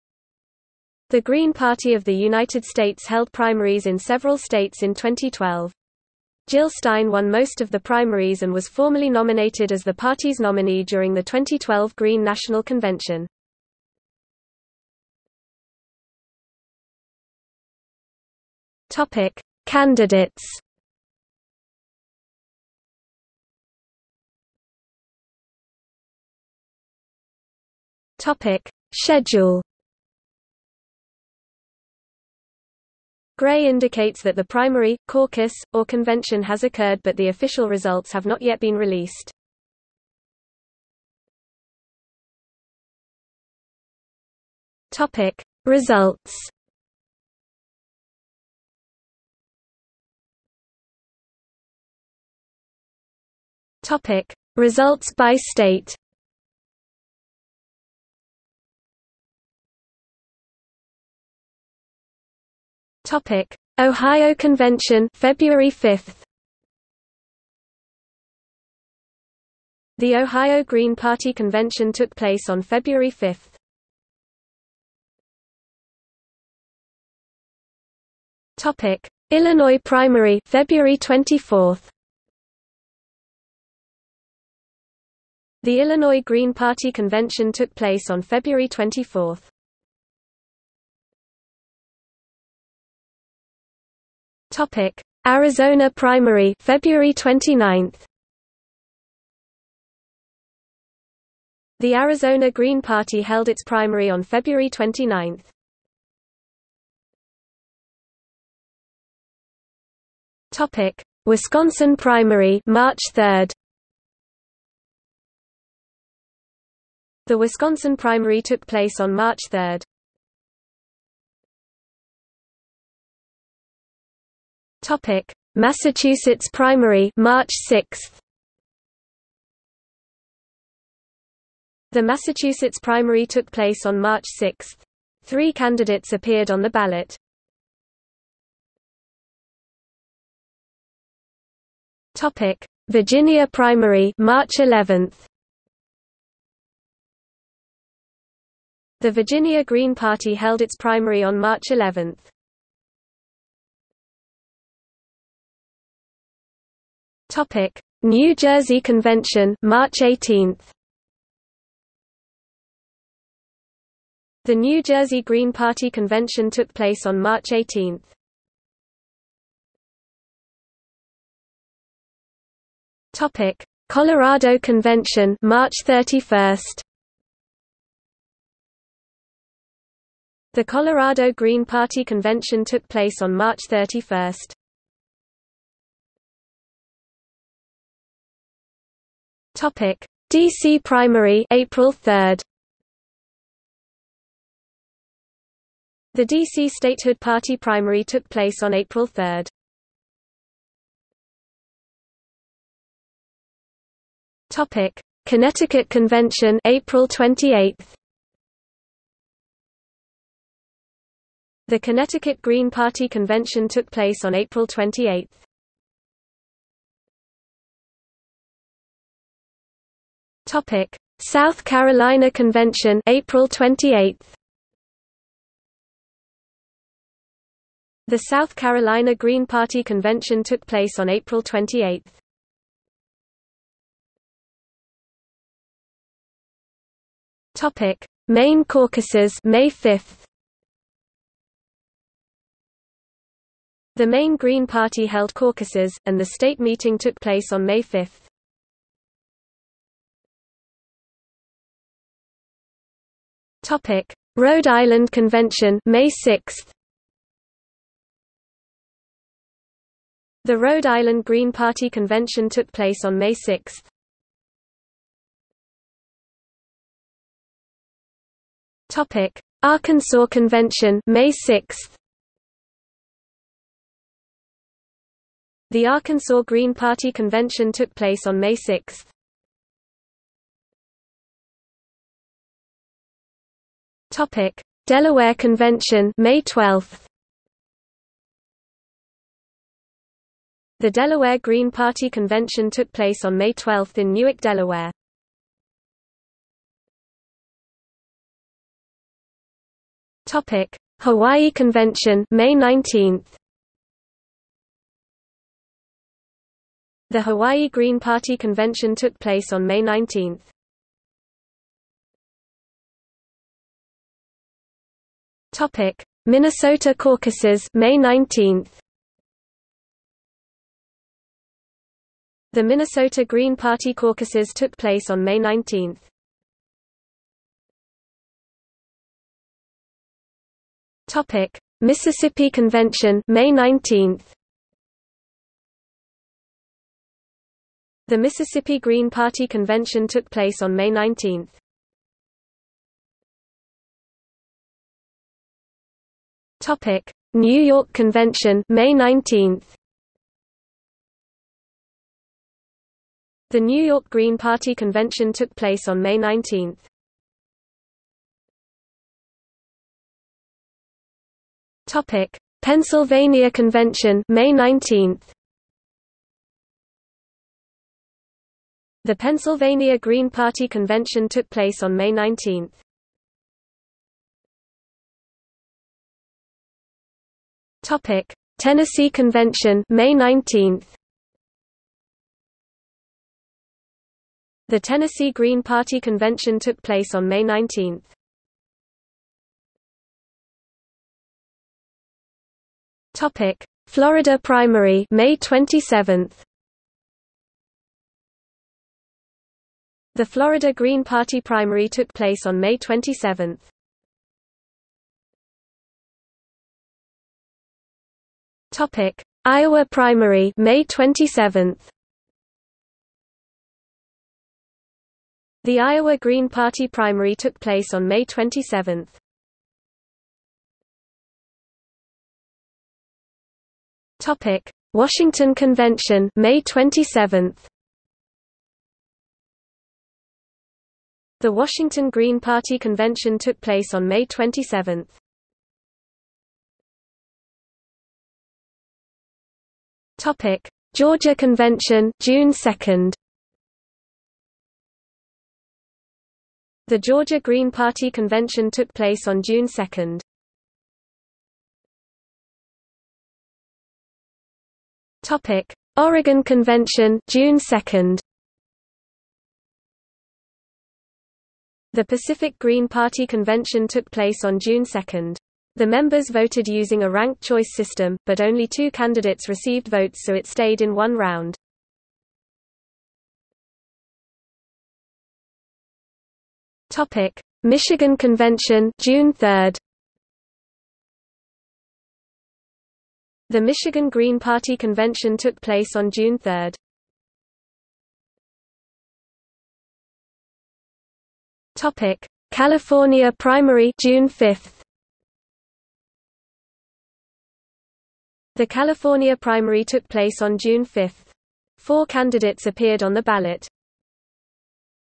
2012 Green Party presidential primaries | Wikipedia audio article
00:00:15 1 Candidates
00:00:24 2 Schedule
00:00:41 3 Results
00:00:50 3.1 Results by state
00:00:59 3.2 Ohio Convention (February 5)
00:01:15 3.3 Illinois primary (February 24)
00:01:31 3.4 Arizona primary (February 29)
00:01:47 3.5 Wisconsin primary (March 3)
00:02:01 3.6 Massachusetts primary (March 6)
00:02:18 3.7 Virginia primary (March 11)
00:02:33 3.8 New Jersey convention (March 18)
00:02:49 3.9 Colorado convention (March 31)
00:03:06 3.10 DC Primary (April 3)
00:03:21 3.11 Connecticut convention (April 28)
00:03:37 3.12 South Carolina convention (April 28)
00:03:54 3.13 Maine Caucuses (May 5)
00:04:10 3.14 Rhode Island convention (May 6)
00:04:25 3.15 Arkansas convention (May 6)
00:04:40 3.16 Delaware convention (May 12)
00:04:56 3.17 Hawaii convention (May 19)
00:05:12 3.18 Minnesota Caucuses (May 19)
00:05:27 3.19 Mississippi convention (May 19)
00:05:43 3.20 New York convention (May 19)
00:05:58 3.21 Pennsylvania convention (May 19)
00:06:14 3.22 Tennessee convention (May 19)
00:06:29 3.23 Florida primary (May 27)
00:06:45 3.24 Iowa primary (May 27)
00:07:00 3.25 Washington Convention (May 27)
00:07:16 3.26 Georgia convention (June 2)
00:07:31 3.27 Oregon convention (June 2)
00:07:53 3.28 Michigan convention (June 3)
00:08:08 3.29 California primary (June 5)

- Socrates

SUMMARY
The Green Party of the United States held primaries in several states in 2012. Jill Stein won most of the primaries and was formally nominated as the party's nominee during the 2012 Green National Convention.